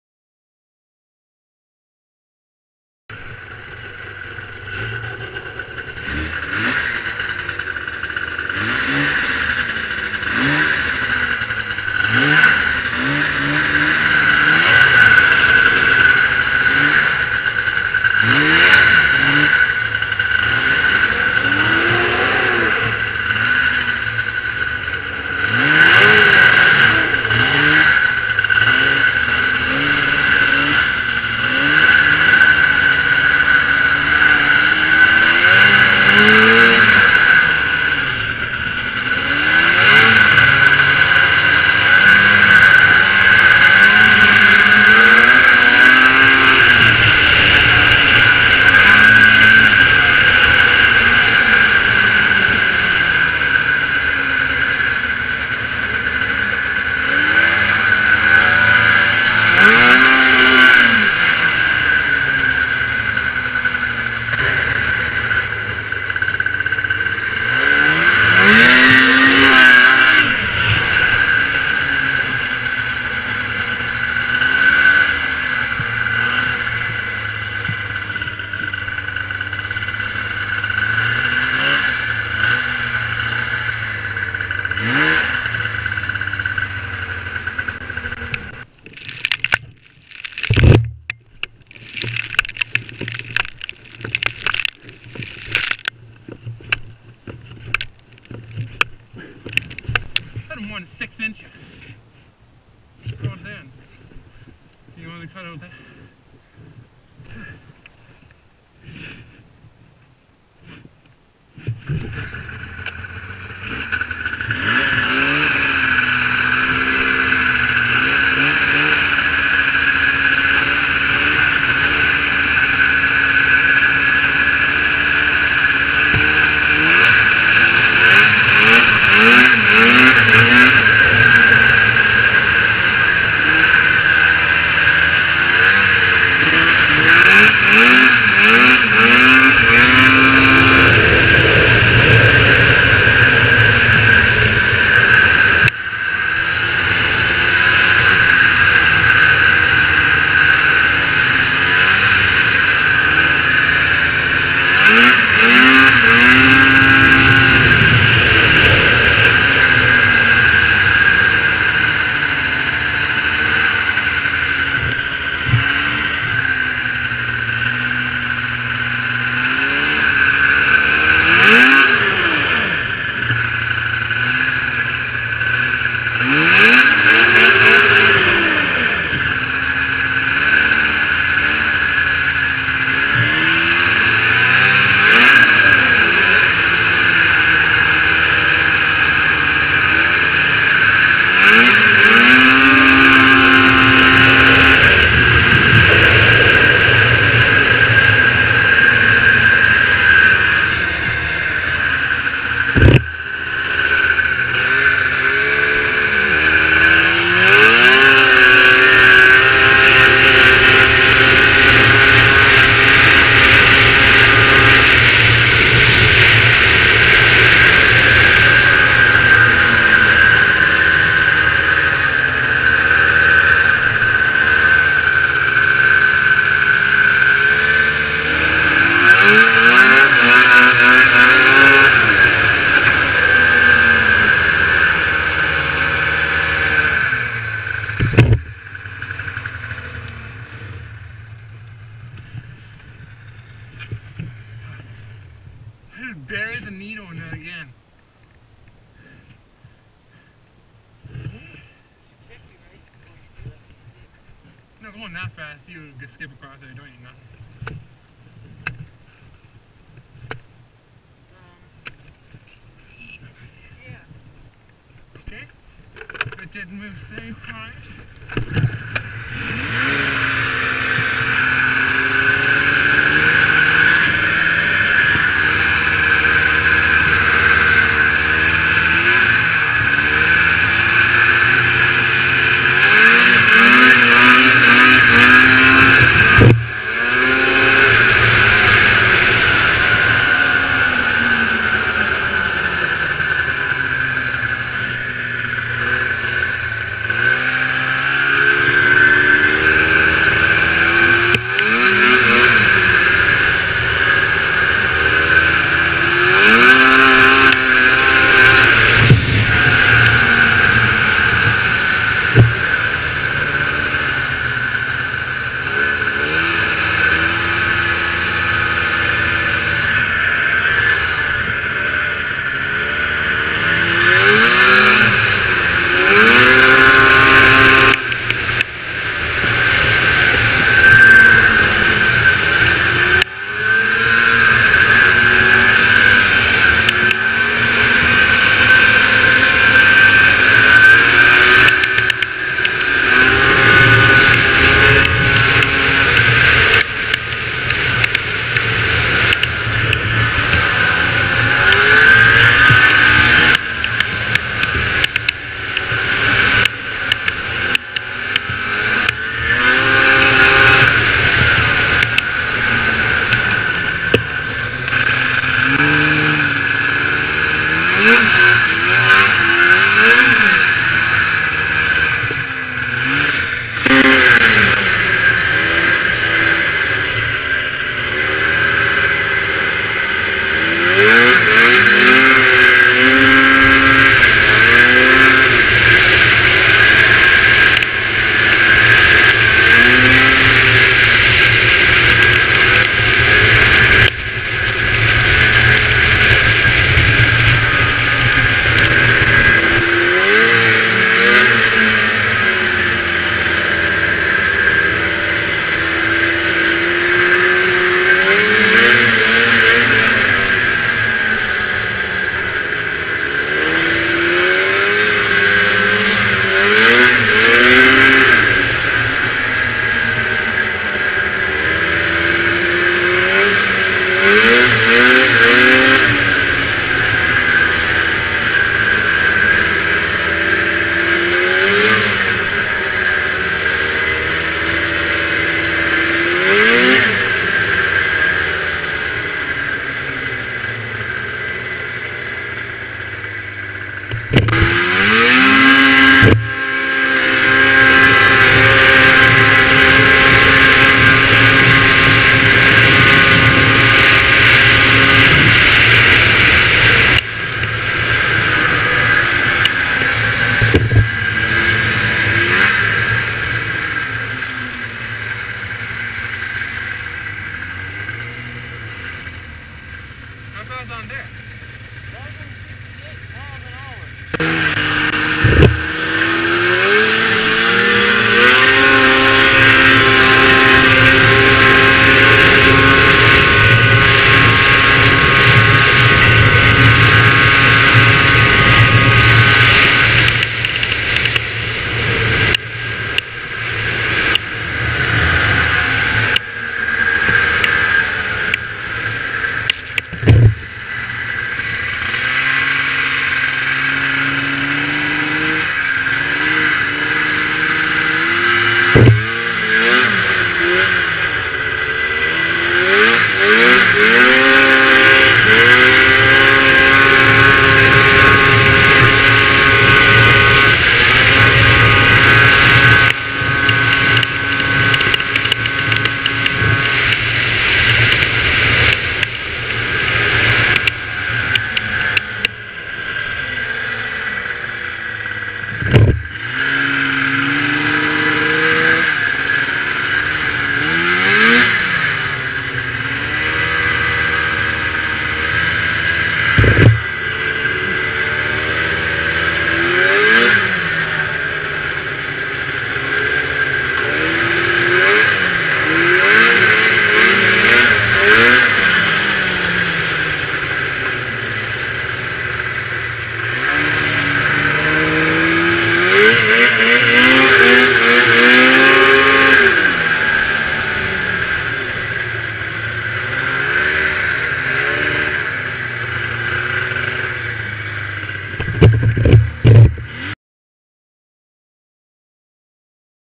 out for a ride on wildcat 650cc
im on my 1988 arctic cat wildcat 650cc and my friends sled is 2006 polaris fusion 700